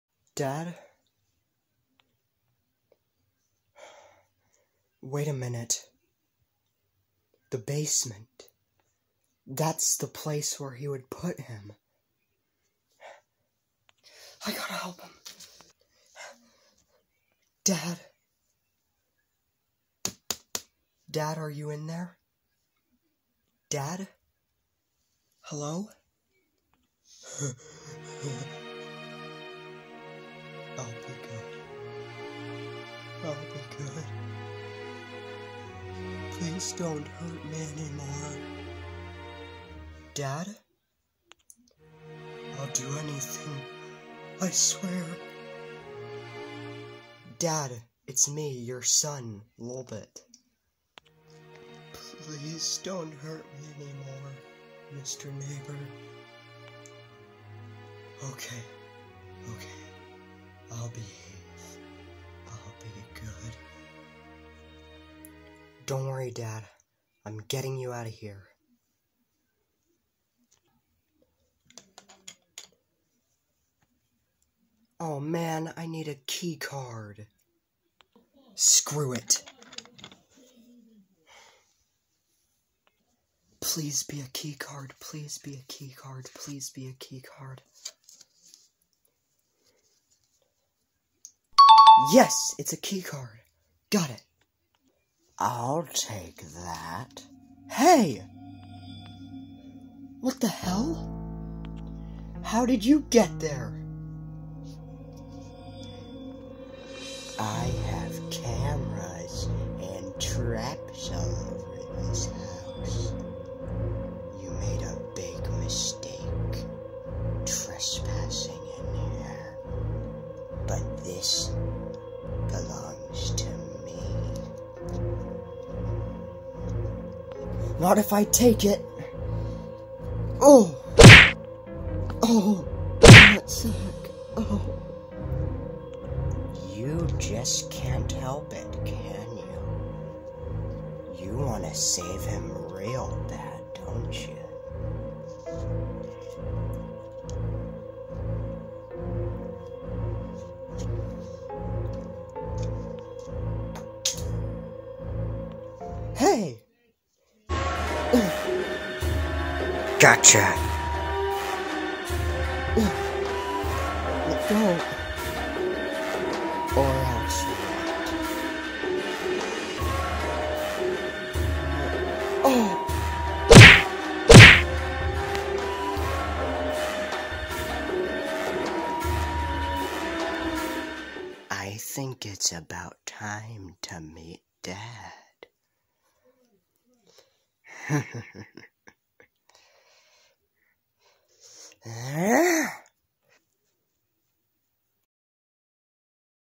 HELLO NEIGHBOR PLUSH EPISODE 2: THE NEIGHBOR RETURNS! (PART 2)
LOLBIT CONTINUES TO TRY TO SAVE HIS FATHER AFTER THE NEIGHBOR KICKED LOLBIT OUT OF HIS HOUSE, BUT THE NEIGHBOR INTERVENES AND KEEPS THE KEY CARD FROM HIM BEFORE CATCHING AND TAKING HIM TO SEE HIS DAD! LIKE AND SUB FOR PART 3!